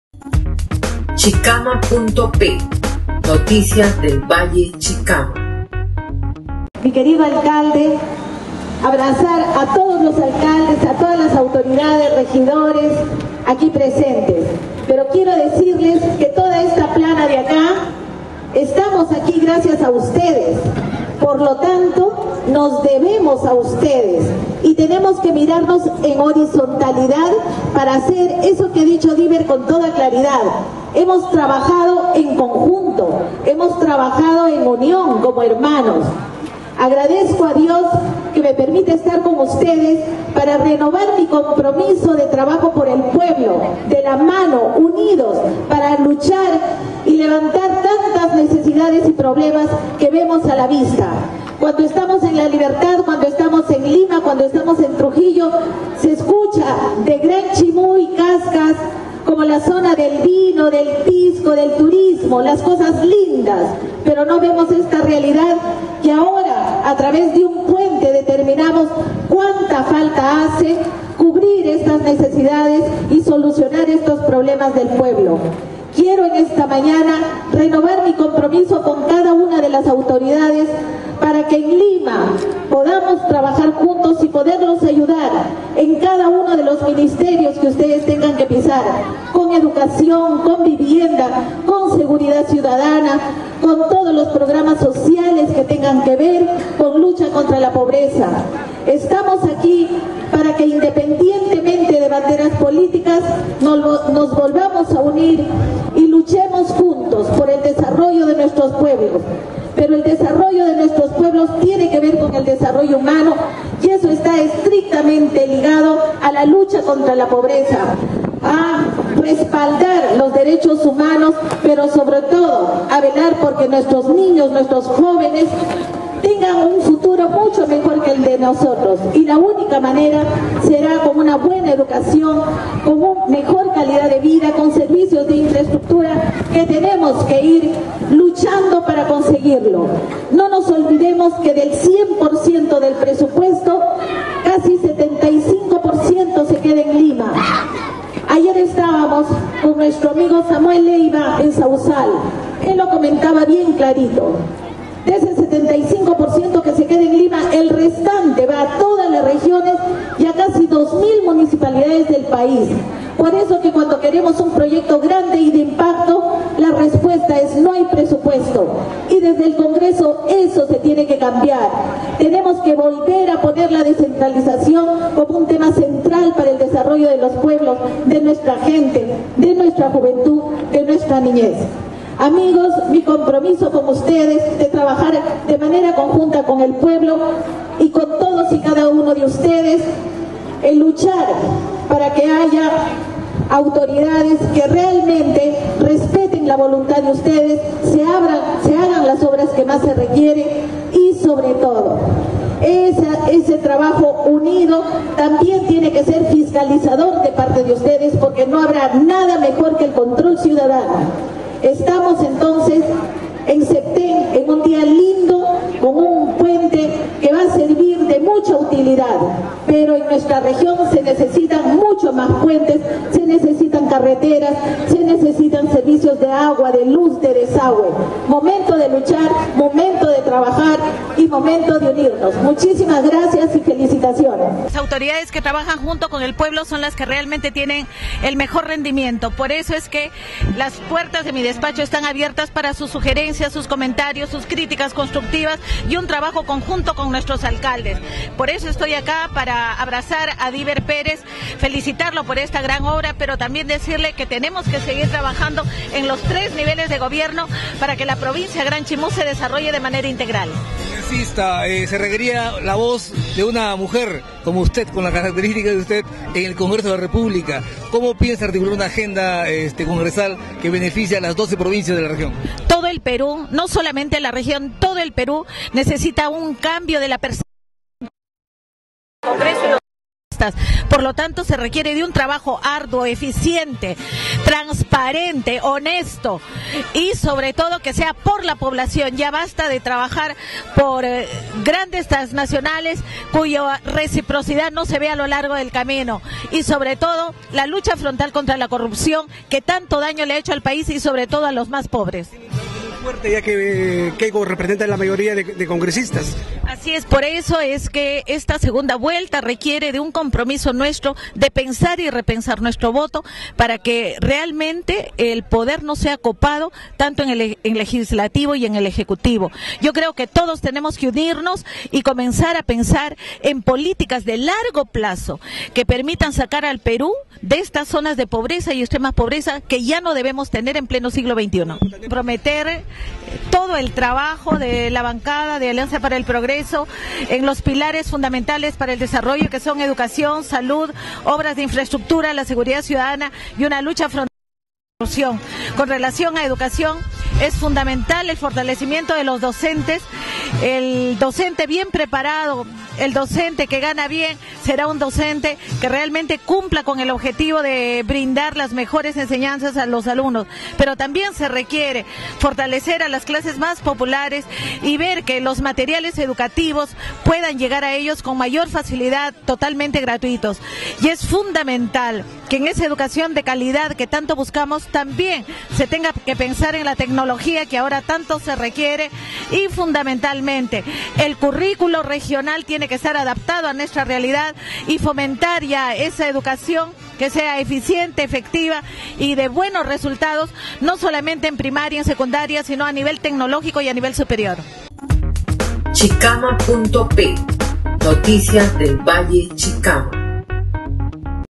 Gloria Montenegro Madrina del Puente Colgante Septen
Congresista electa Gloria Montenegro Figueroa - Madrina del Puente Peatonal Colgante Septen.